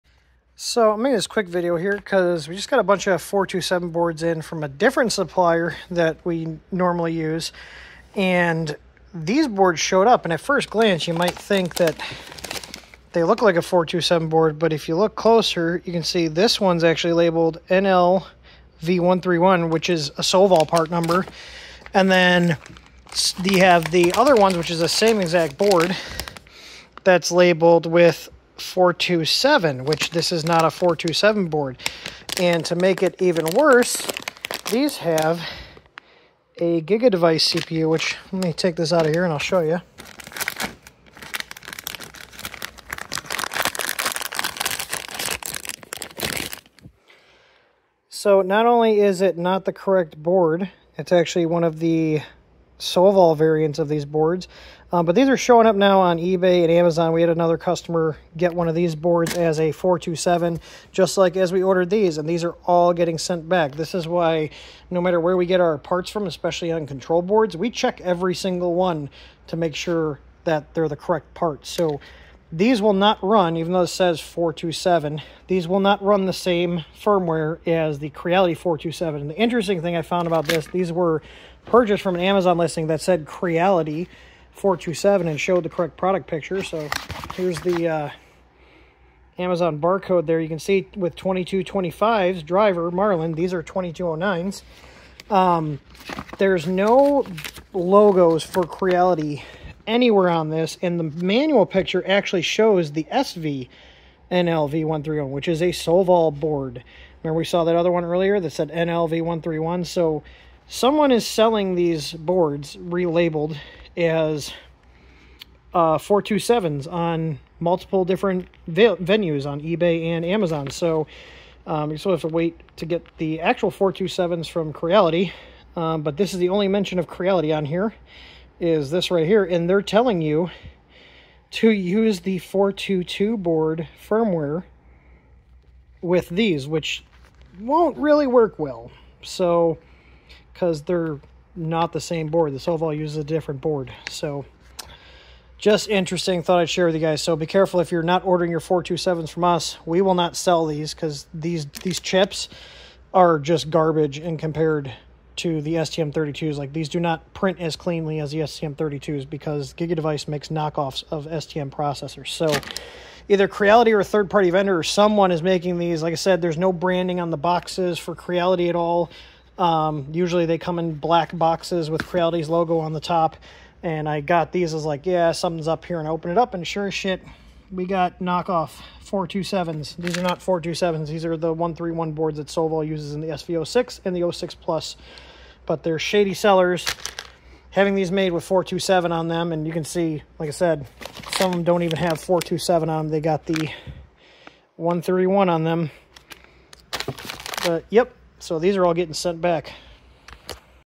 Fake V4.2.7 Boards Amazon & Ebay - Gigadevice CPUs & Completely Different Board
🎥 We create videos and live streams for our community.